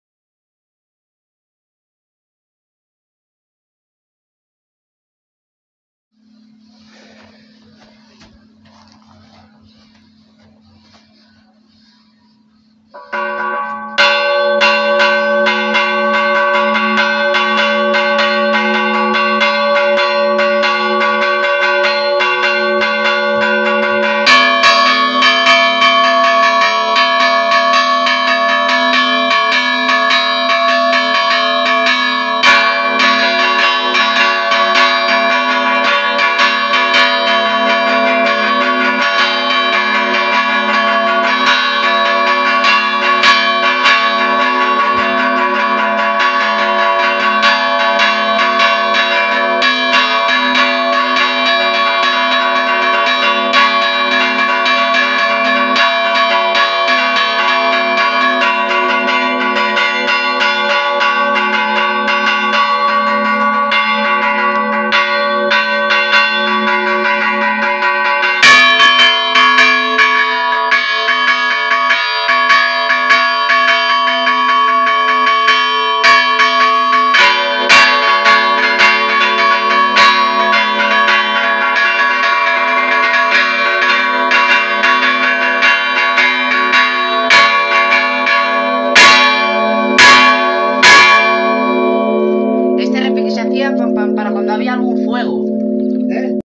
Toque a Rebato en Castrillo Mota de Judíos
El pasado 15 de Agosto de 2022 por la festividad de la Asunción de la Virgen (y la víspera de San Roque) tuvo lugar una muestra de toques en la iglesia románica de San Esteban en el pueblo de Castrillo Mota de Judíos a cargo del campanero Ulises Hernando.

*Estoy muy agradecido con los del pueblo, sobretodo Antonino (Fue él el que nos dejó realizar los toques de las campanas incluso recuperar el toque manual). Y con otros chicos del pueblo que estuvieron con nosotros*

Al principio de cada vídeo se especifica:
1: nombre del intérprete
2: toque de campanas
3: población de cual procede el toque de campanas